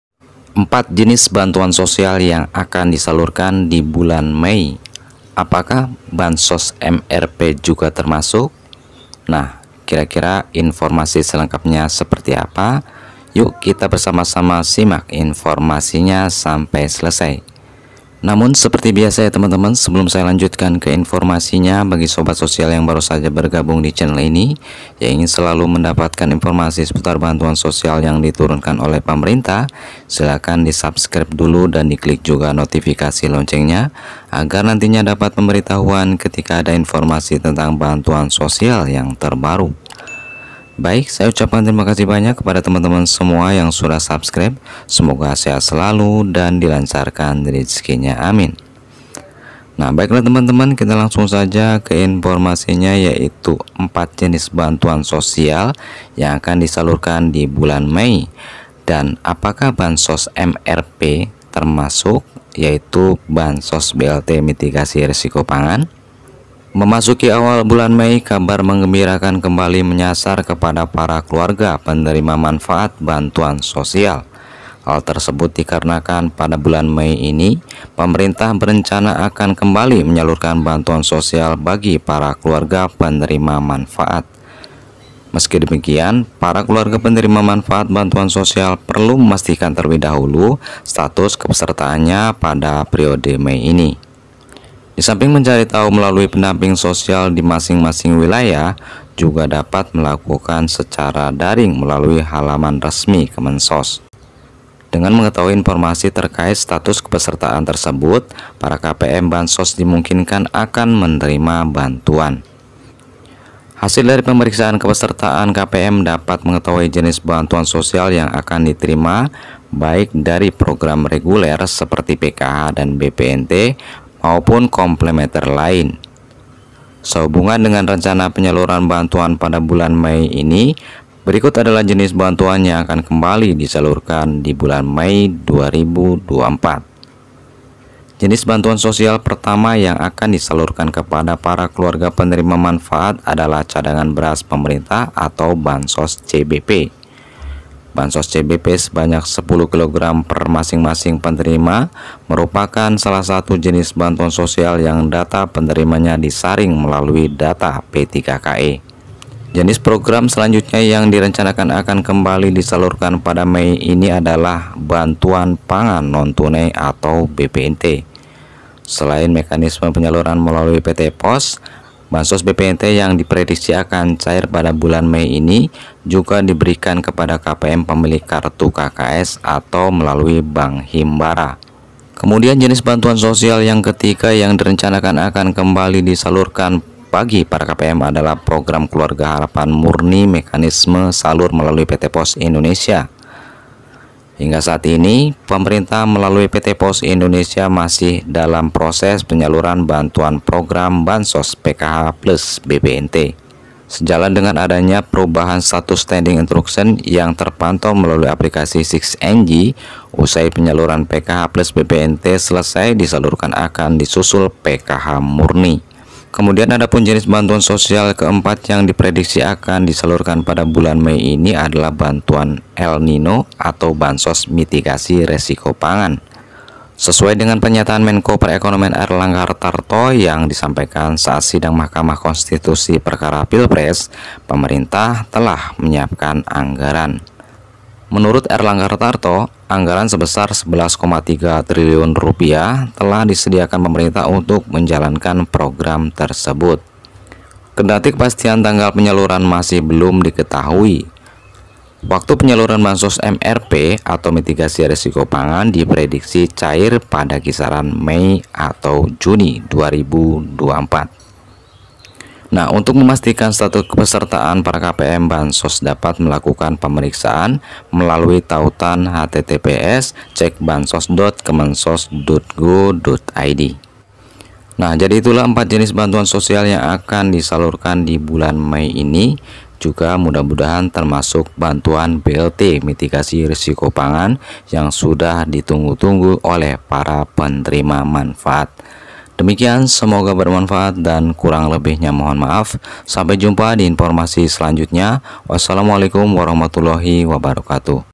4 Jenis Bantuan Sosial yang Akan Disalurkan di Bulan Mei, Bansos MRP Termasuk?
4 Jenis Bantuan Sosial yang Akan Disalurkan di Bulan Mei, Bansos MRP Termasuk?